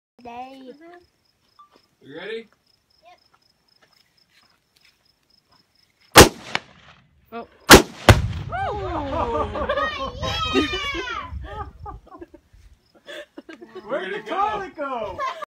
Blowing up a Toilet with a Tannerite Explosive Target
We set a 1 lb Tannerite Explosive Target on top of our old toilet and shot it from 100yds away. You can see it pretty much dusted the whole toilet, there were only a few bowling ball sized pieces left.